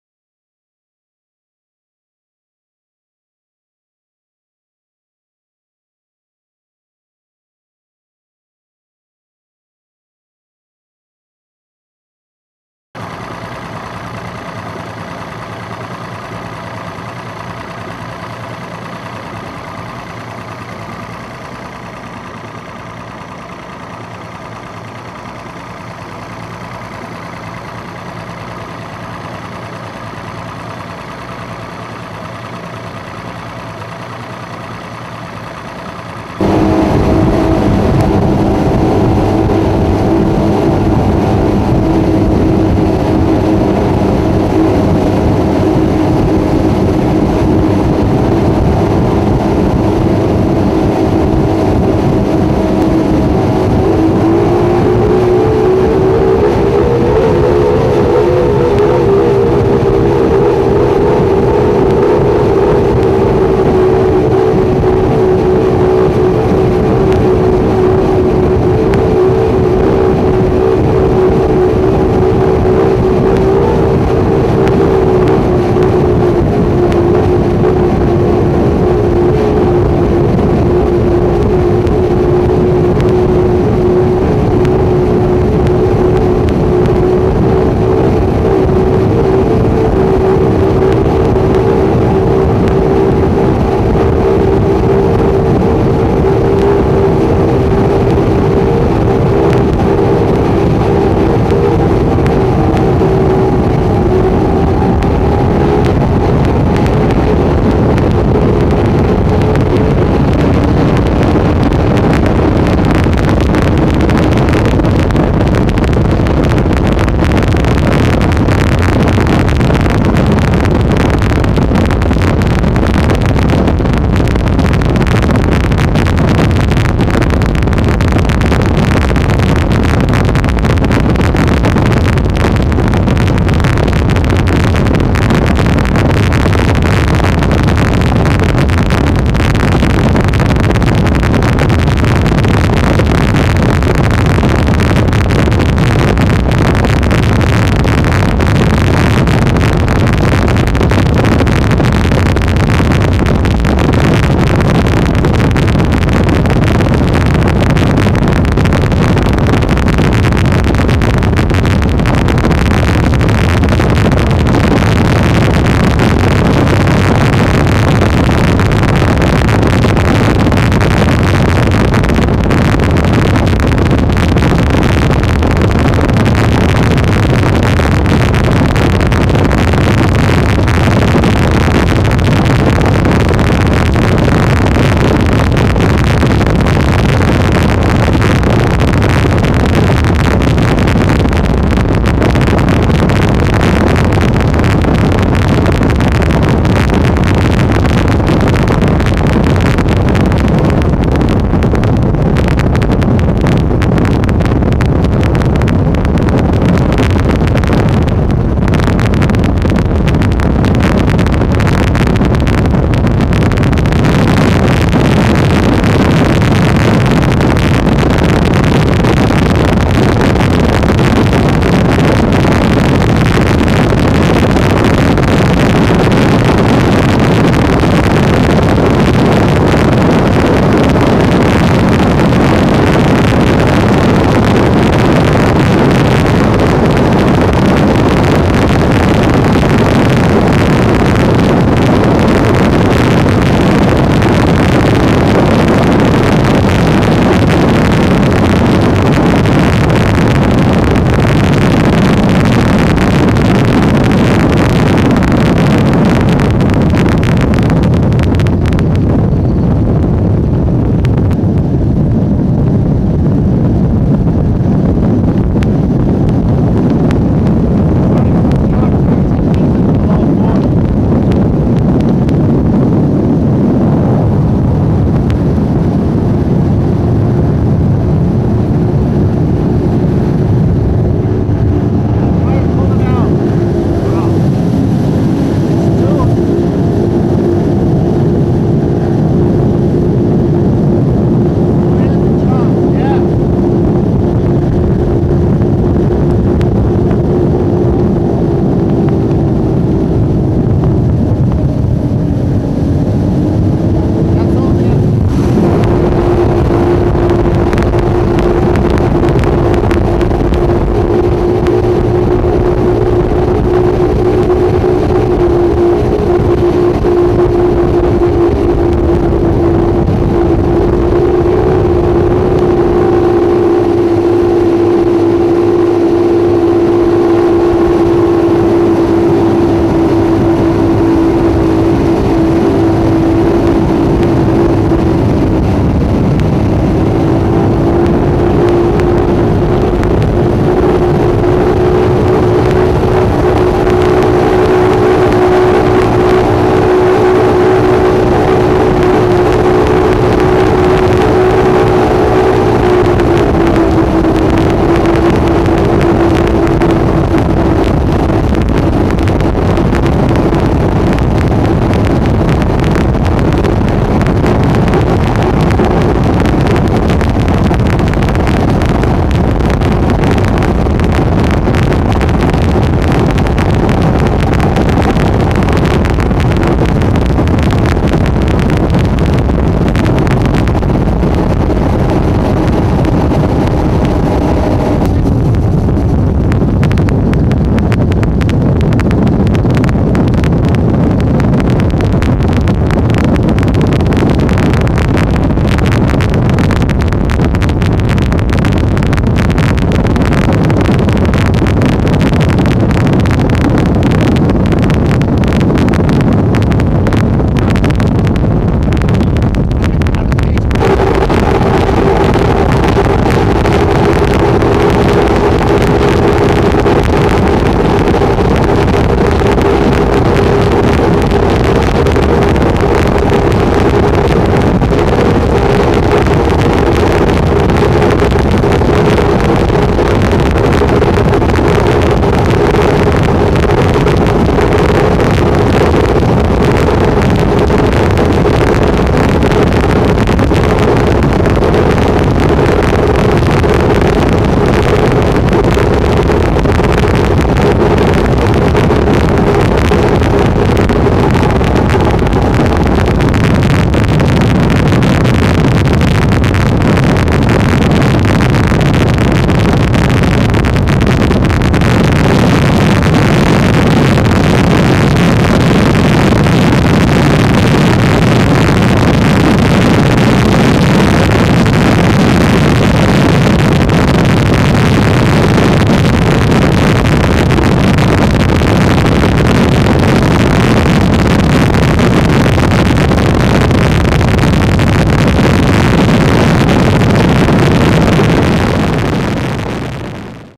First Run of Skater on Cayuga Lake
Rons first Run of new Skater on Cayuga Lake - over the transom filming at speed